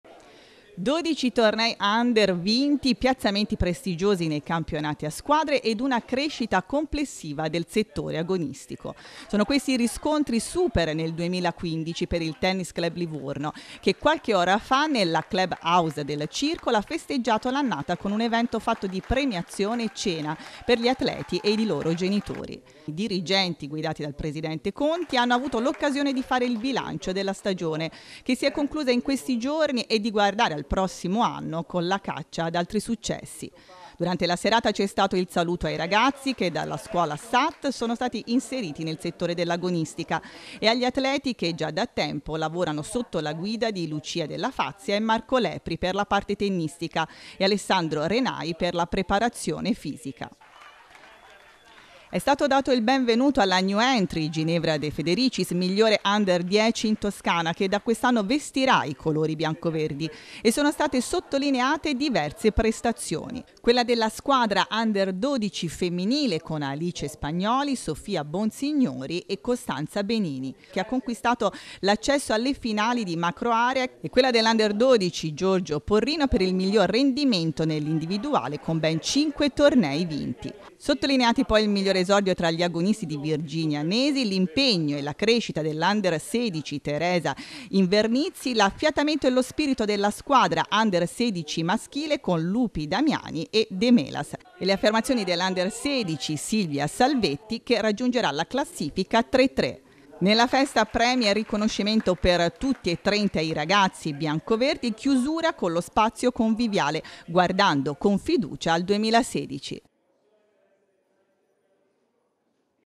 FESTA AL TC LIVORNO
Servizio di Granducato TV sulla festa al Tennis club Livorno per i risultati del settore Under